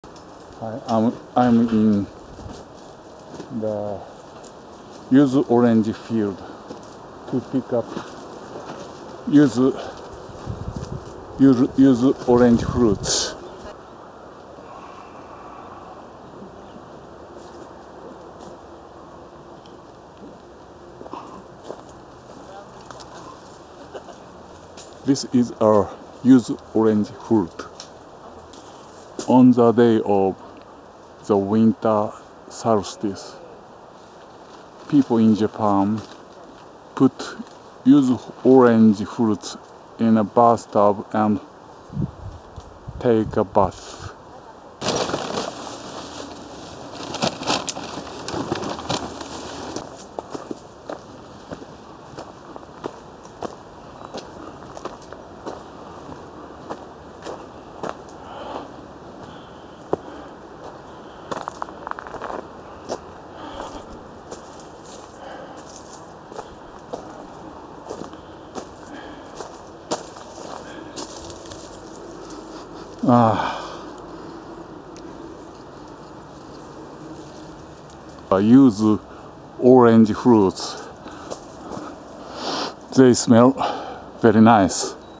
Picking yuzu orange fruits ゆずもぎ
Ozawa Syuzo sake brewery owns a yuzu orange field. They recruited volunteers to pick yuzu orange fruits, so I gave a hand to help them. After the work, they gave me a lot of fruits. I'll put some of these fruits in my bathtub to enjoy "yuzu-yu."
小澤酒造さんが所有するゆず畑で、ゆず捥ぎをするのに、ボランティアを募集していたので、ちょっとお手伝いをしてきました。捥いだゆずを分けてもらいましたので、今度、お風呂に入れてゆず湯を楽しむ予定です。